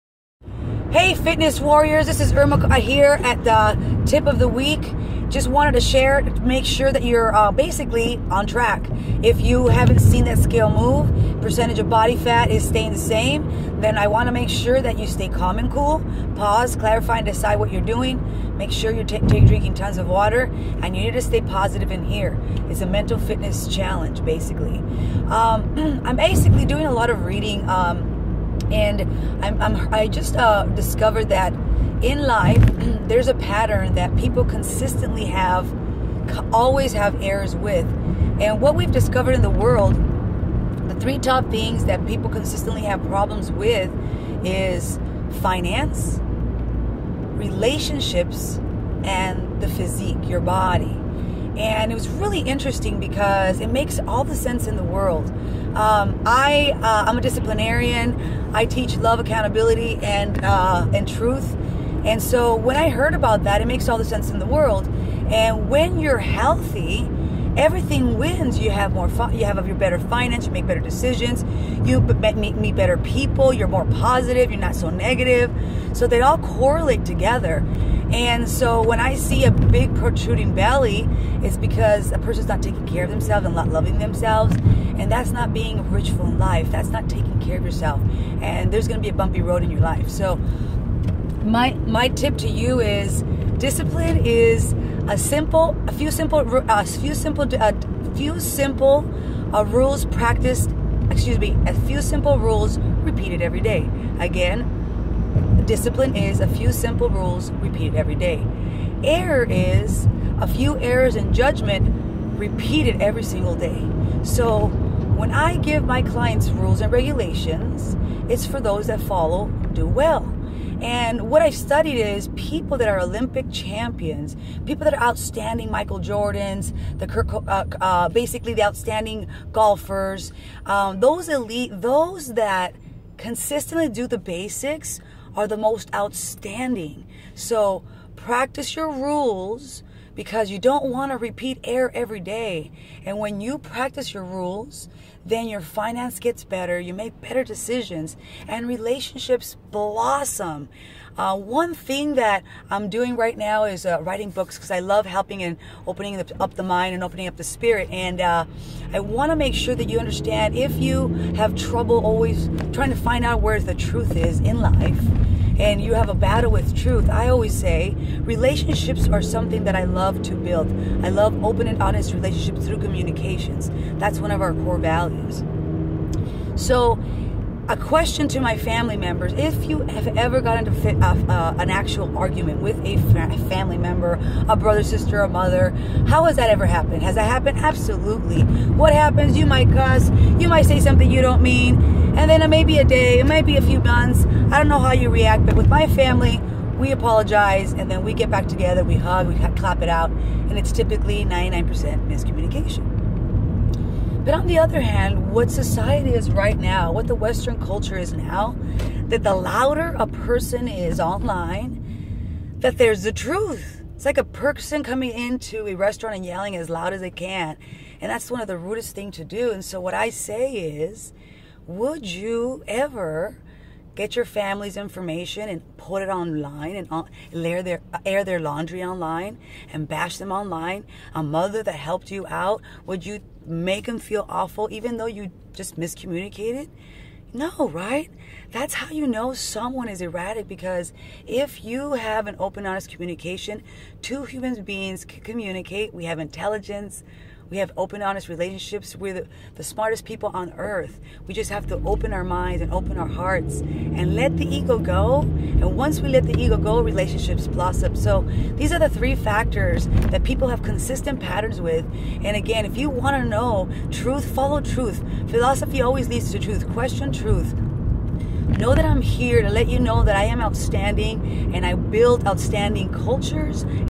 Personal Trainer Reveals the 3 Problems People Have in life
1. Finance
2. Relationships
3. Body